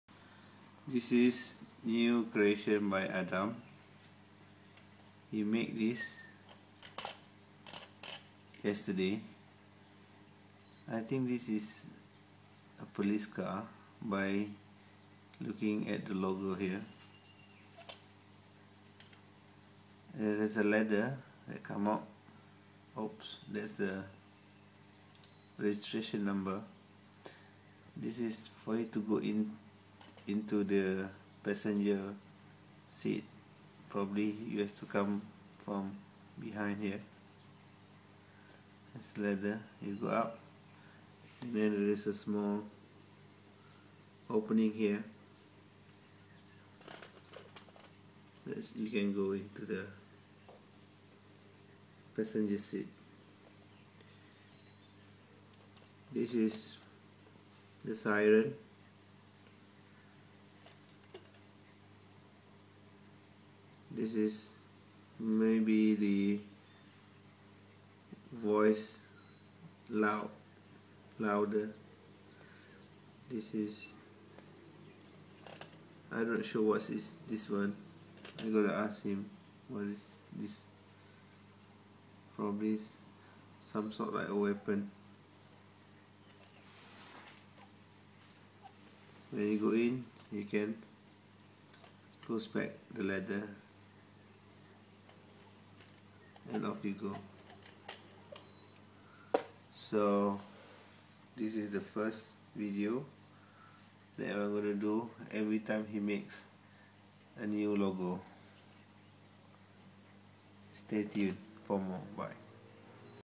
LEGO Futuristic police car
Made by my son, Adam who is a 7 years old dyslexic.